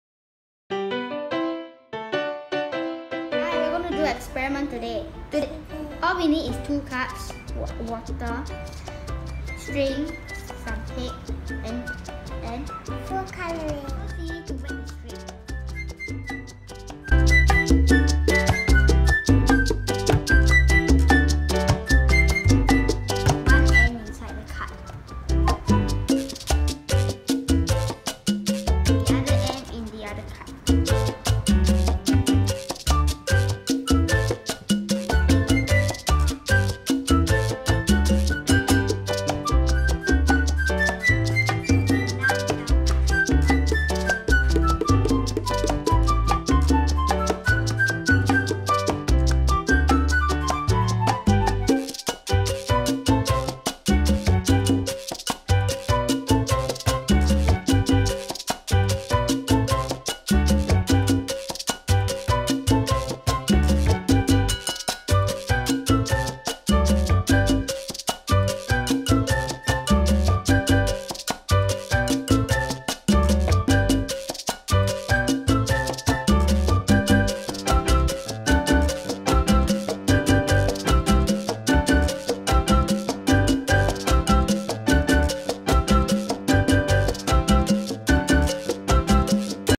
Science Experiment: Travelling water!
A science experiment to see how water travels! Amazing discovery for the kids and adults!